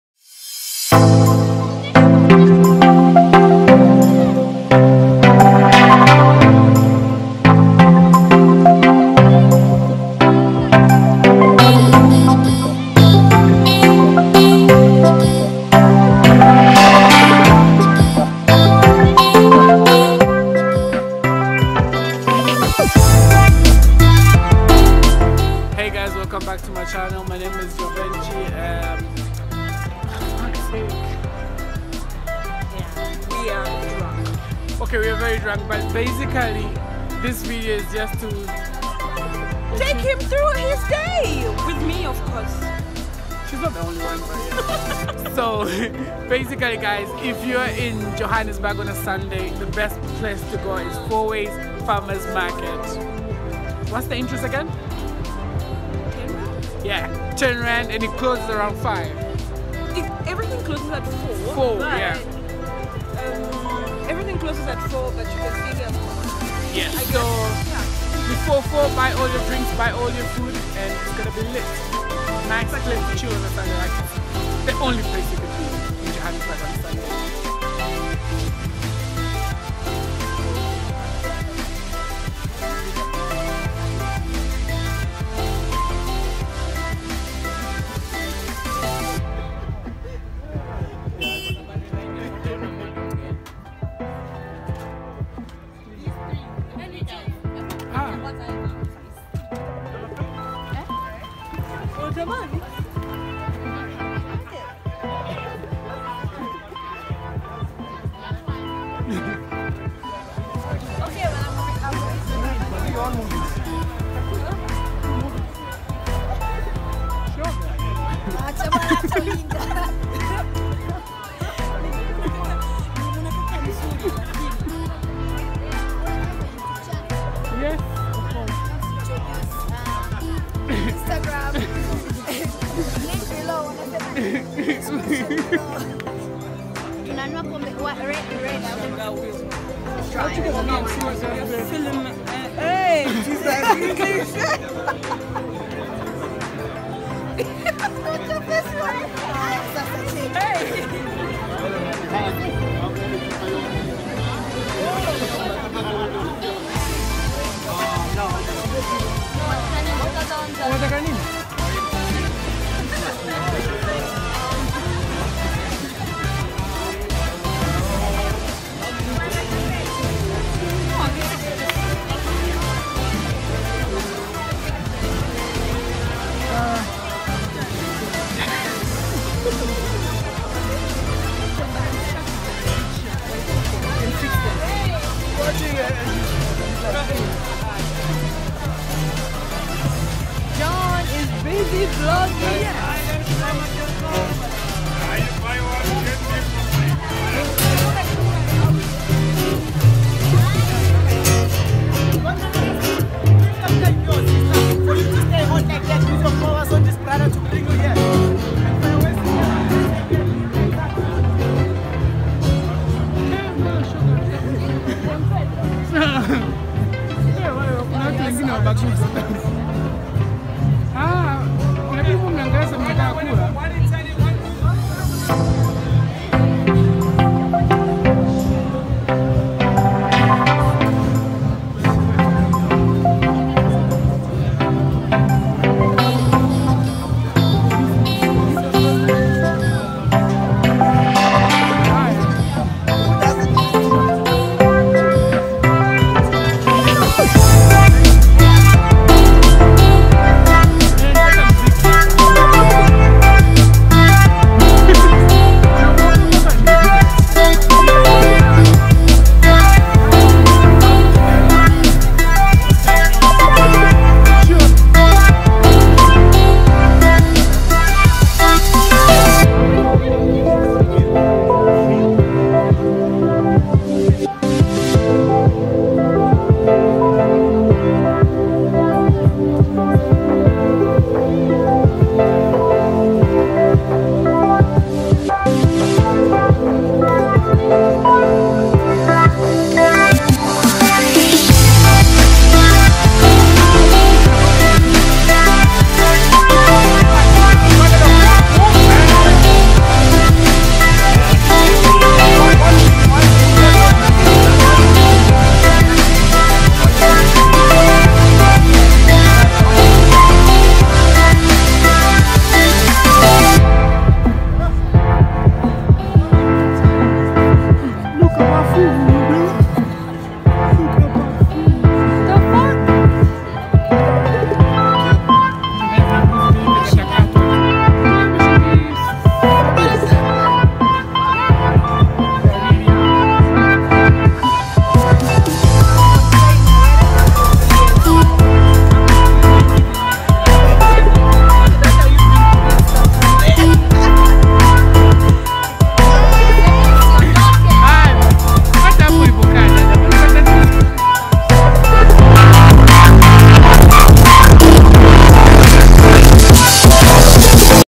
EXPLORING FOURWAYS FARMER MARKET | JOHANNESBURG
Fourways Farmers Market Is A Beautiful Outdoor Market That’s Is Located On The Real Fourways Farmstead .

This Market Has About 107 Vendor Stalls Selling Selected Prpducts That Are Artisanal And Delicious As Well .Theyll Serve You Food From Award Wining Chefs To Experienced Marketers Who Are Passionate About Their Products And Specialists In Their Field As well .

The Market Entrance Tickets And Shop Shutdown At 4  So Get There Early And Get your Food And Drinks On Your arable  Before Four